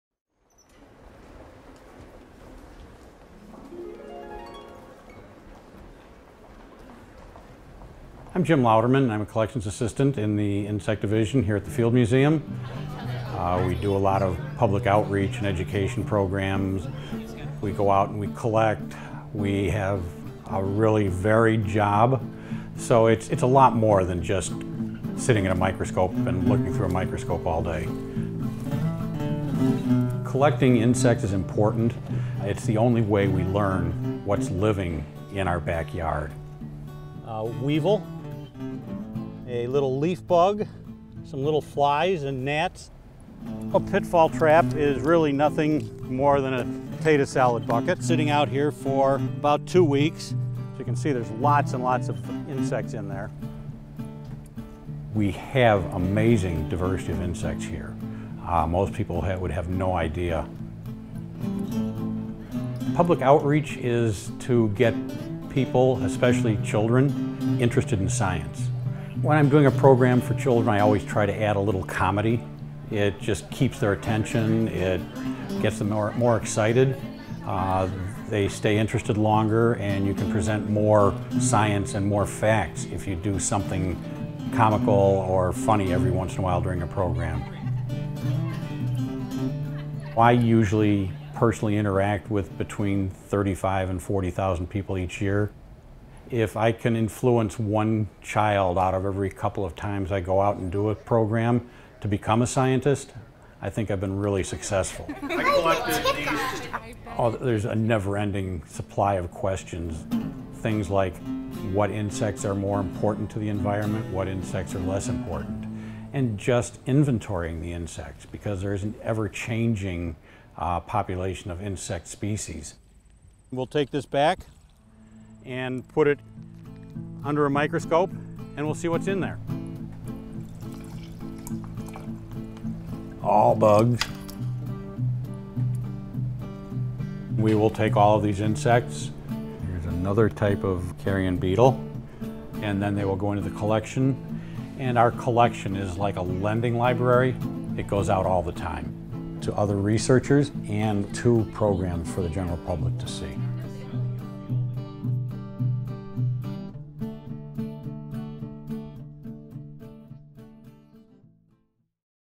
The Man Who Mistook His Tarantula for a Hat
Collections Assistant Jim Louderman wears many hats (some of which are tarantulas). In addition to preparing specimens for the Field Museum's Insect Collection, he collects insects and arachnids around Illinois and the central U.S., and participates in numerous public outreach programs.